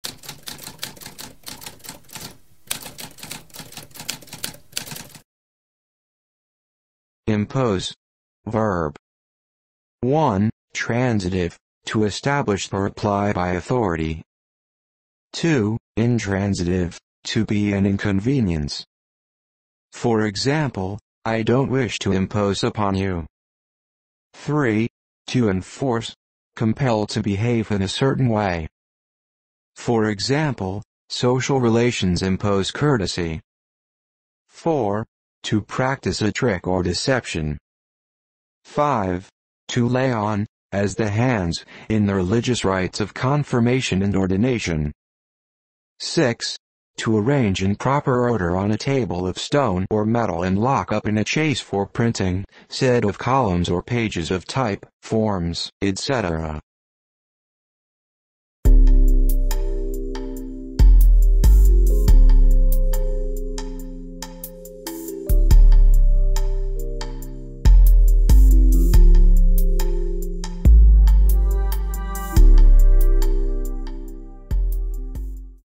What does impose mean?
A spoken definition of impose.

Intro Sound:
Typewriter - Tamskp